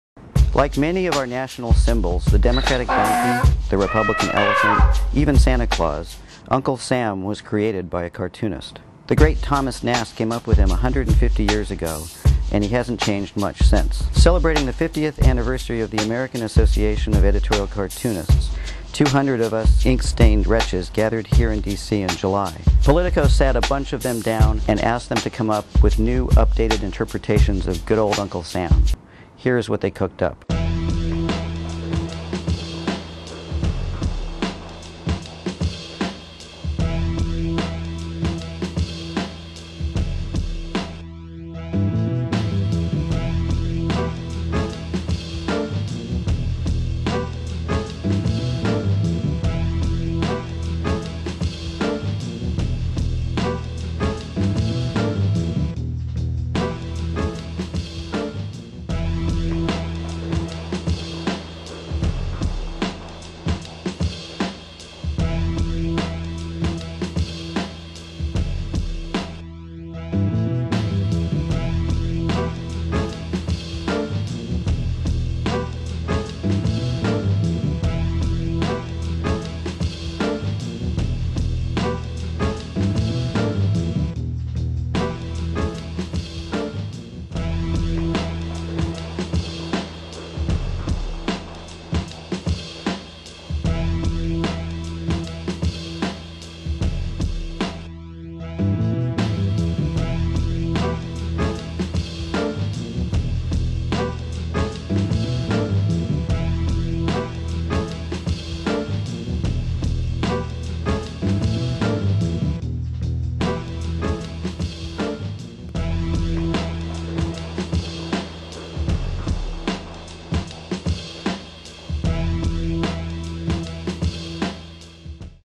Drawing Uncle Sam
Celebrating the 50th Anniversary of the American Association of Editorial Cartoonists, 200 artists attended a convention in Washington, D.C. Politico asked a handful of them...Can you draw Uncle Sam?